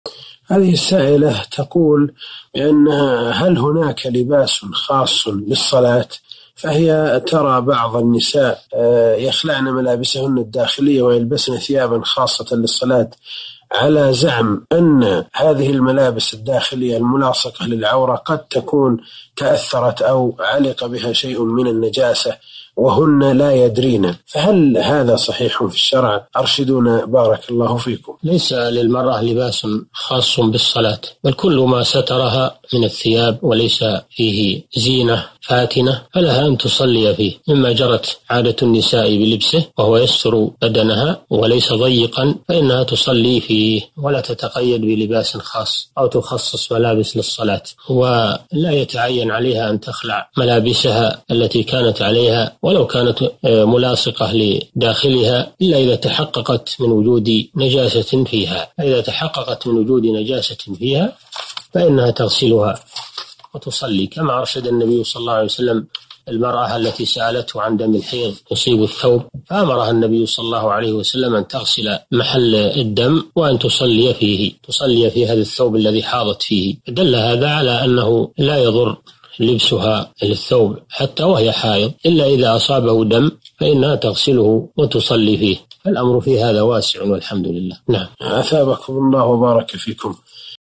هل هناك لباس خاص للصلاة؟ الشيخ صالح بن فوزان الفوزان
نص السؤال:
تقول هل هناك لباس خاص للصلاة لأنها ترى بعض النساء يخلعن ملابسهن الداخلية ويلبسن ثياباً خاصة للصلاة على رعم أن هذه الملابس الداخلية الملاصقة للعورة قد تكون تأثرت أو علق فيها شيء من النجاسة وهن لا يدرين فهل هذا صحيح في الشرع أرشدونا بارك الله فيكم؟

السيرة الذاتية :
للاشتراك في قناة الشيخ صالح الفوزان

رئيس الفريق
لمشروع كبار العلماء